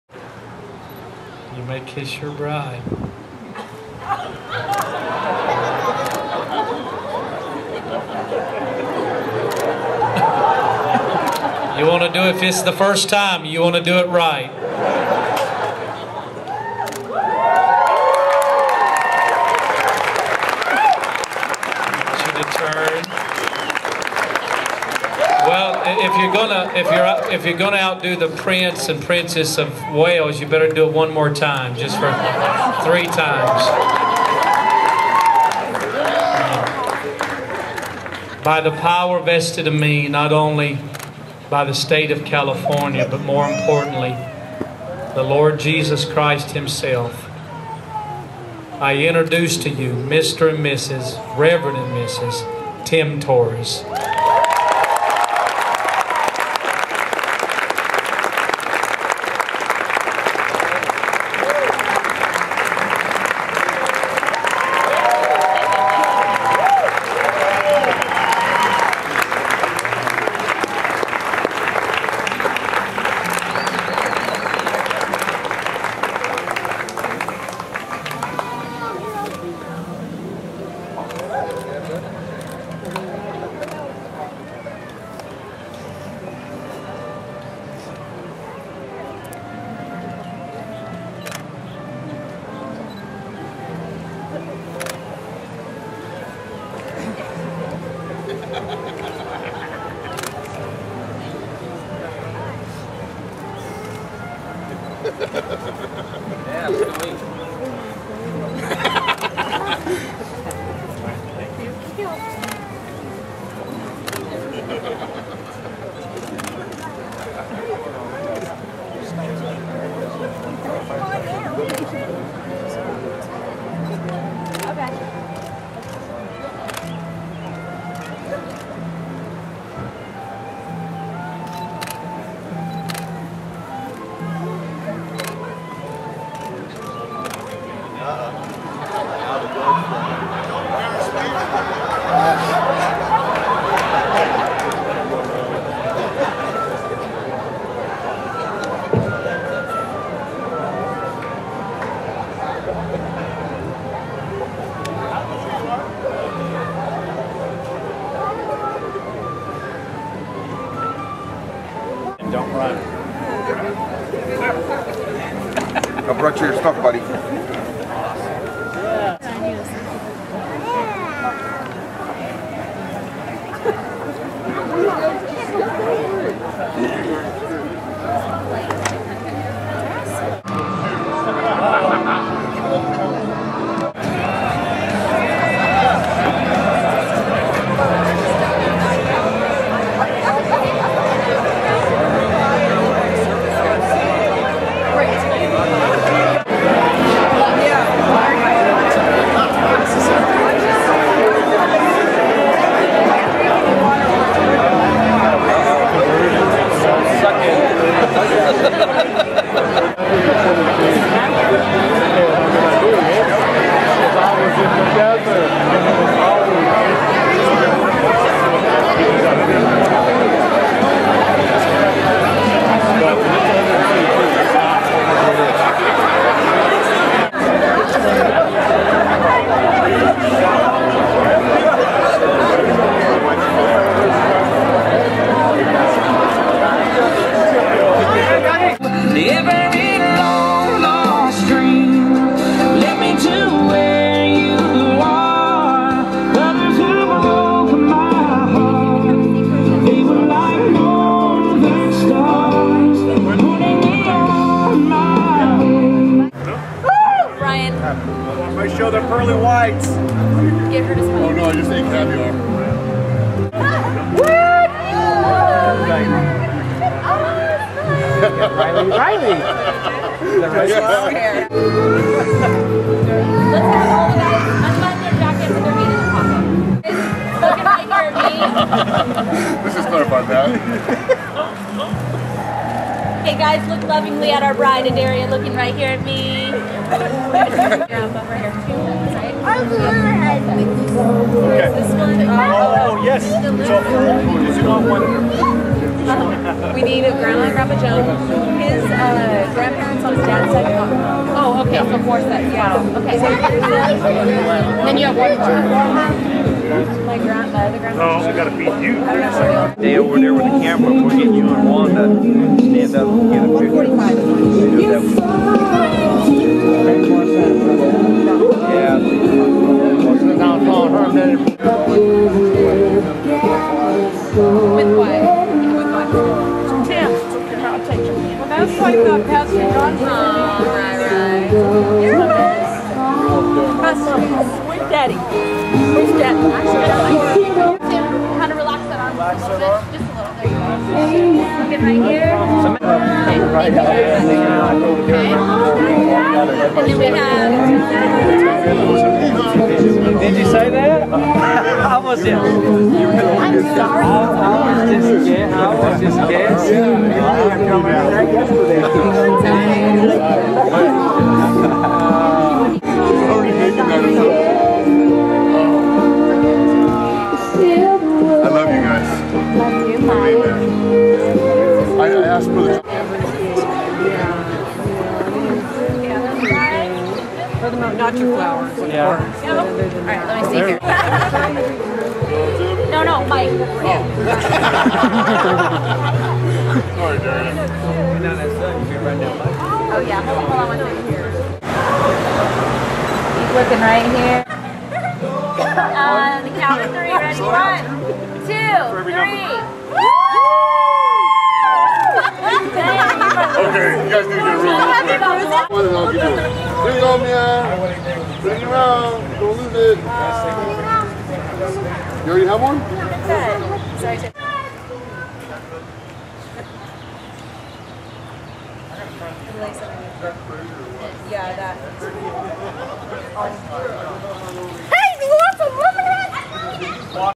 Wedding Kiss at our Royal Wedding
A Royal Wedding that mean something. In contrast to the Kim Kardashian wedding, this wasn't a staged publicity stunt.  Our beautiful wedding was at Wine and Roses, Lodi, CA. The Kiss, Pronouncement, and Wedding Pictures with family. Todd Johnson from Revival Center of Modesto was the preacher.